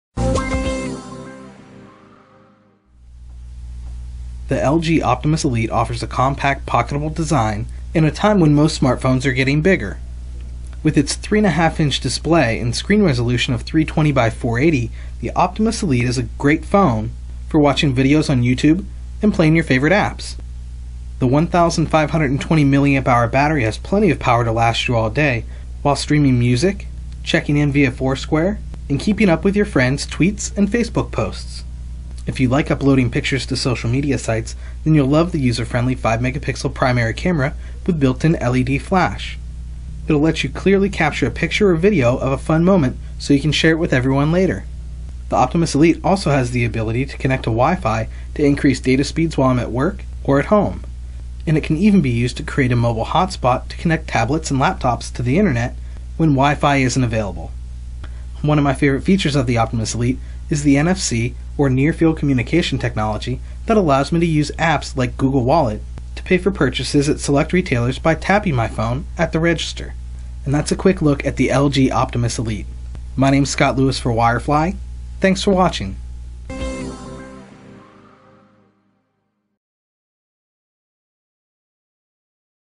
LG Optimus Elite Smartphone Quick Look by Wirefly
The LG Optimus Elite has a compact design that will fit nicely in your purse or pocket while still giving you a 3.5 inch LCD touch screen.

In this Quick Look video, Wirefly's Scott Lewis will give you a demonstration of the most common tasks people perform on their smartphones every day.  My favorite feature of the LG Optimus Elite is the Near Field Communication technology that allows me use apps like Google Wallet to make purchases in stores.

Need help picking a phone? Use our buyers guide to help find your next phone